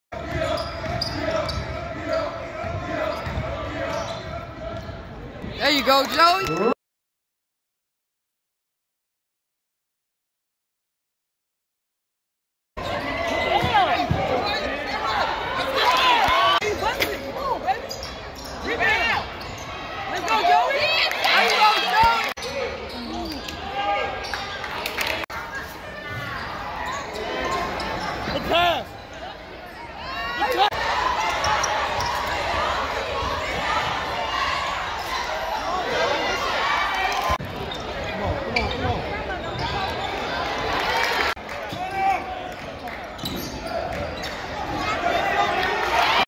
RICH EAST 2020 6'4 F JOEY COKER IS TUFF ON BOTH ENDS OF THE COURT
High school basketball rich East